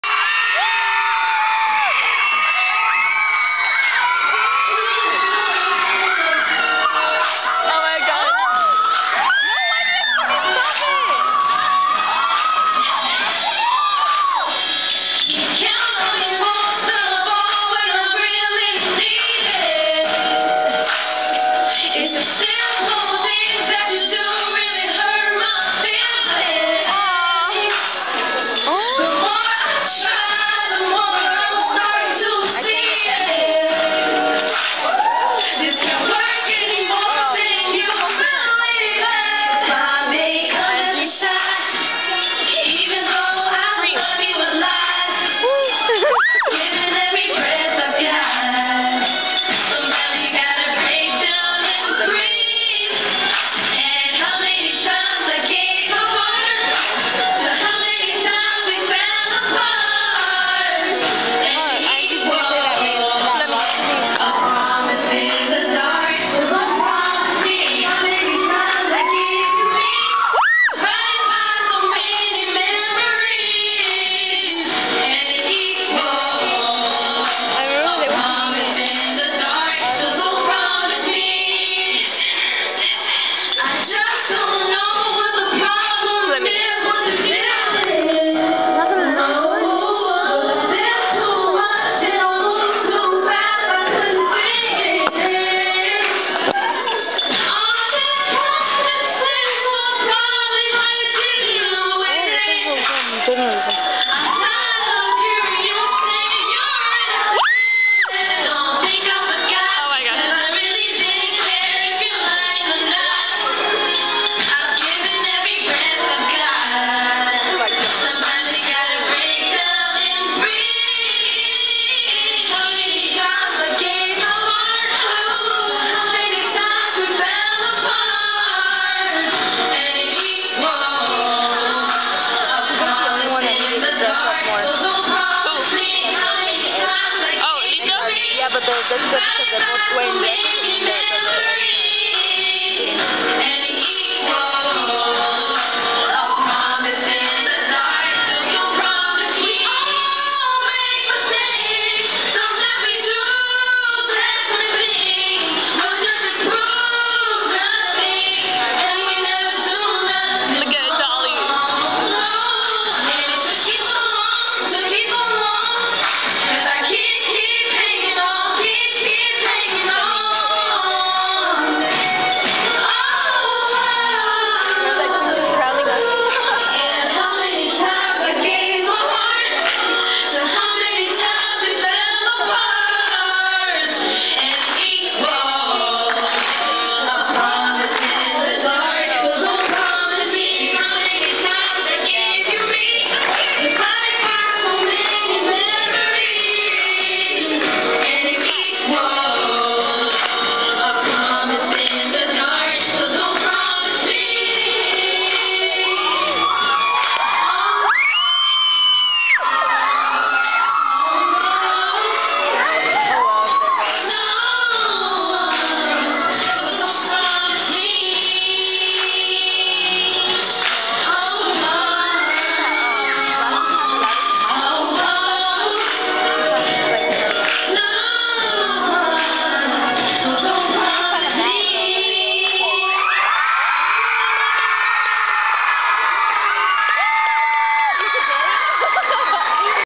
Talent Show 09'
Angie, dolley, rachany, & litha singing for the talent show. (:
they was BOMBB.
OHHH sorry if you didnt see them singing & it was all black.
it was cause the teachers where coming thats why.